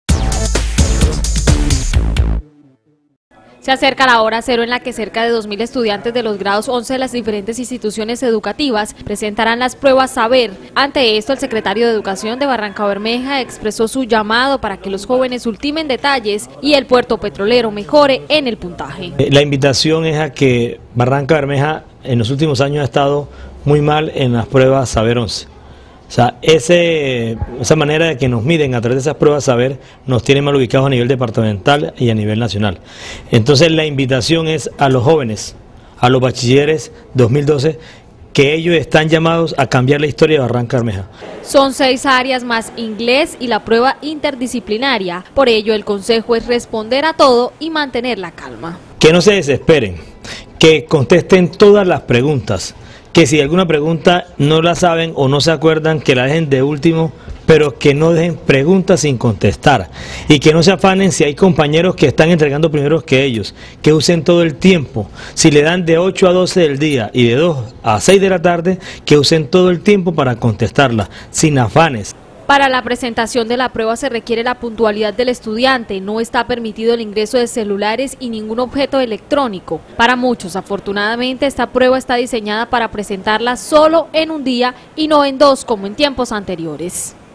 Secretario de educación hace llamado a estudiantes a que se preparen para las pruebas saber 11
Más noticias de Barrancabermeja